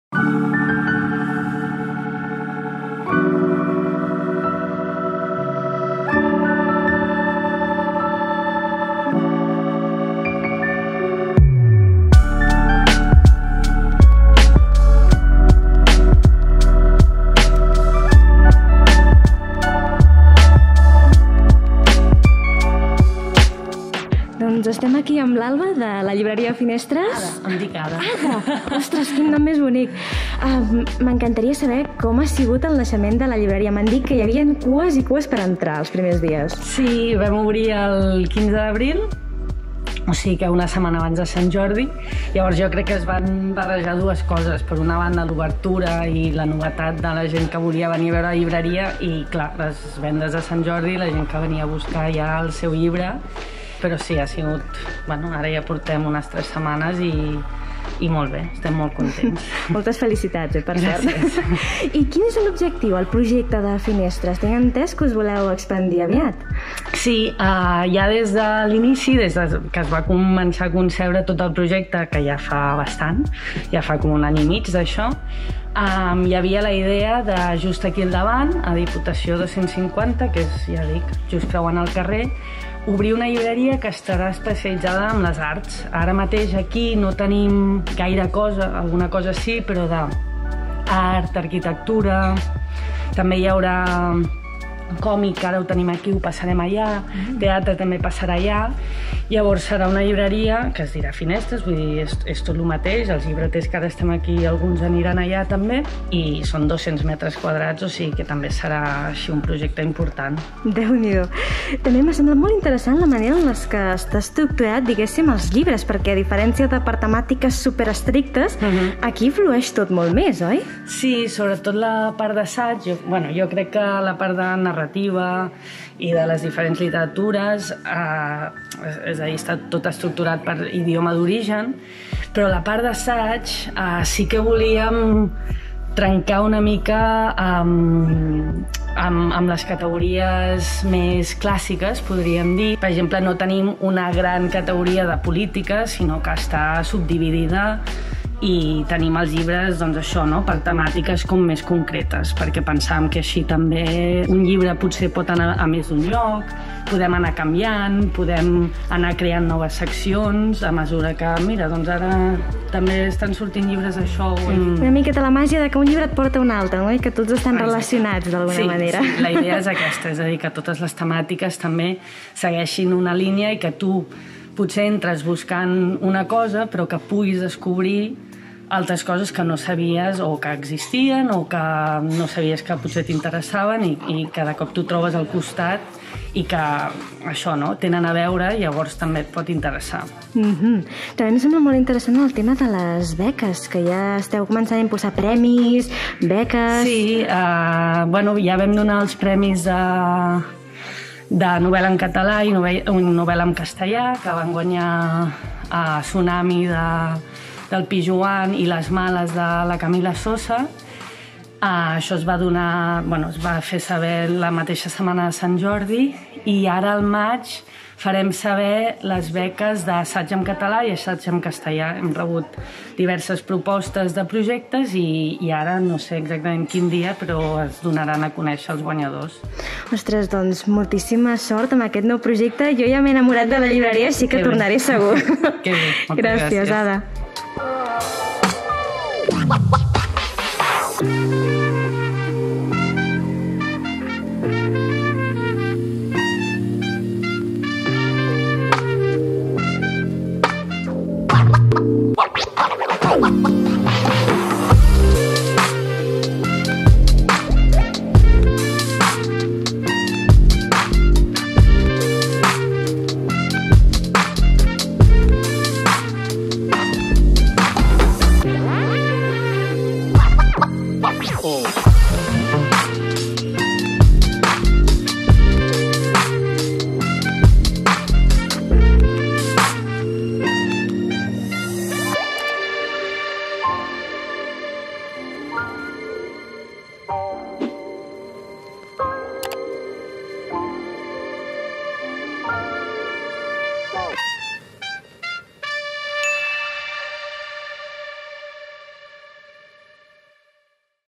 Primera visita a la Llibreria Finestres! (VLOG)
Aquesta setmana he visitat per primera vegada la nova llibreria Finestres i... és fantàstica!!!

Mx